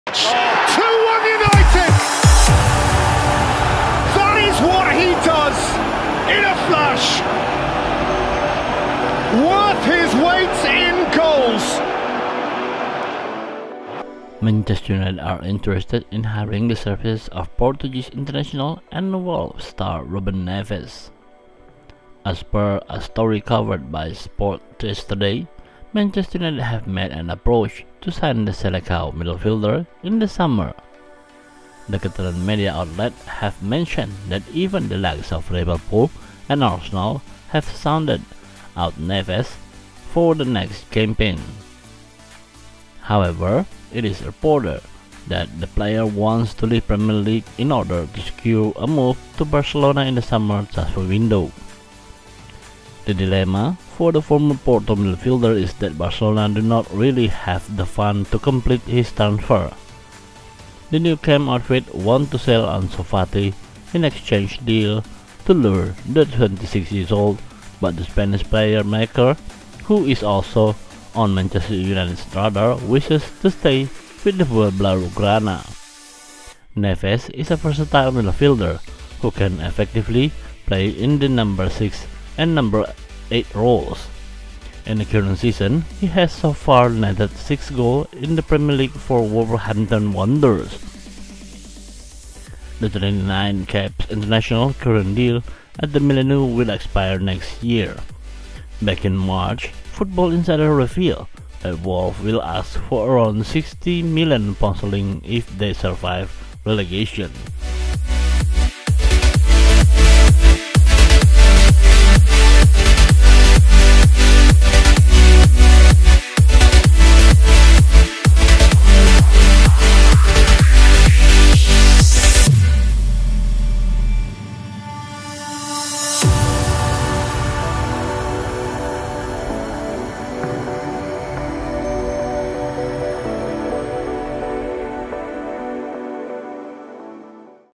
Man Utd have moved in to sign £60million midfielder
Manchester United are interested in hiring the services of Portuguese international and Wolves star, Ruben Neves.
As per a story covered by Sport yesterday , Man Utd have made an approach to sign the Seleccao midfielder in the summer.

The Catalan media outlet have mentioned that even the likes of Liverpool and Arsenal have sounded out Neves for the next campaign.

However, it is reported that the player wants to leave the Premier League in order to secure a move to Barcelona in the summer transfer window.
The dilemma for the former Porto midfielder is that Barcelona do not really have the funds to complete his transfer.

The Nou Camp outfit want to sell Ansu Fati in an exchange deal to lure the 26-year-old but the Spanish playmaker, who is also on Man Utd’s radar, wishes to stay with the Blaugrana.
Neves is a versatile midfielder, who can effectively play in the No. 6 and No. 8 roles. In the current season, he has so far netted 6 goals in the PL for Wolverhampton Wanderers.

The 39-capped international’s current deal at the Molineux will expire next yea. Back in March, Football Insider revealed that Wolves will ask for around £60million if they survive relegation.